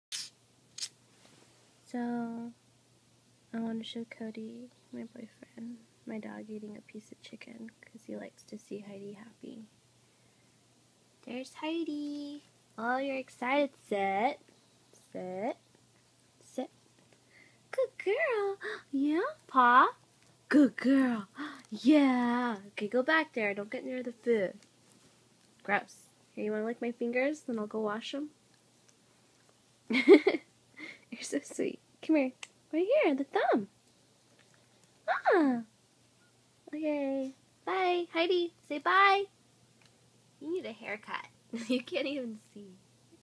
Heidi eating chicken
My dog . A video to show my bf.
I'm sick right now so my voice is weird XD